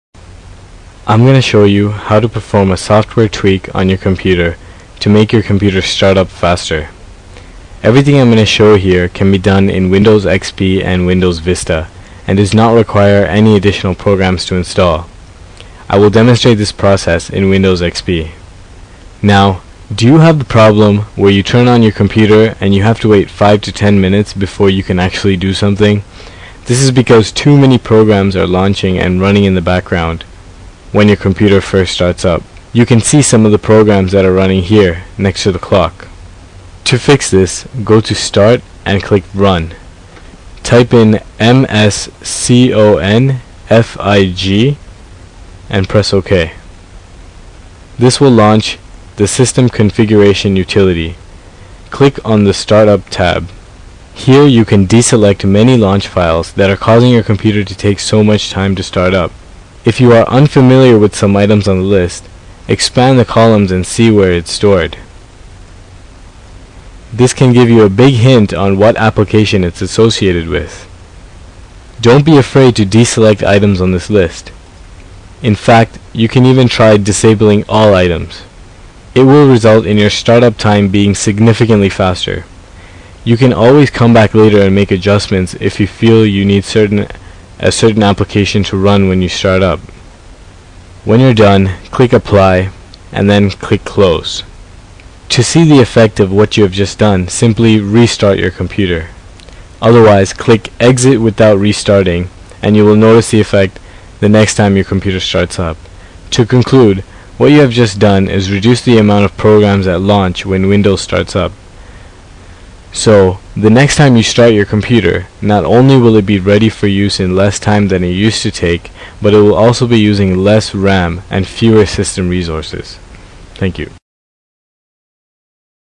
How to make your computer start faster (Windows)
This tutorial explains how to perform a software tweak on your computer to make your computer start up faster. Everything here can be done in Windows XP and Windows Vista, and does not require any additional programs to install.